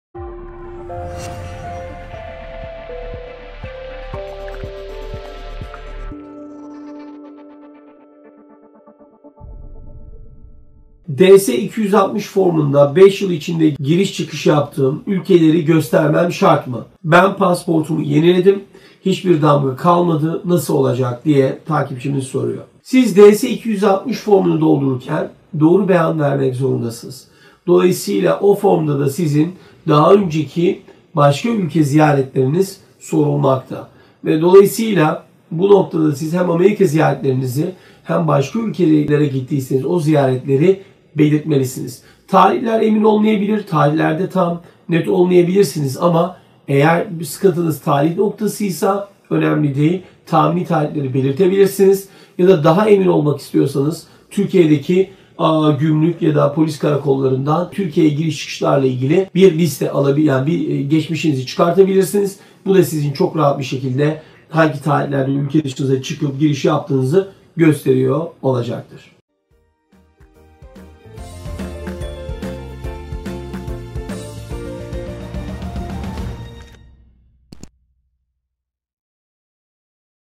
DS260 DORMUNDA GİRİŞ YAPILAN ÜLKE BİLGİLERİ GİRME
Soru: DS260 formunda 5 yıl içinde giriş çıkış yaptığım ülkeleri göstermem şart mı ? Ben pasaportumu yeniledim, hiç bir damga kalmadı, nasıl olacak ?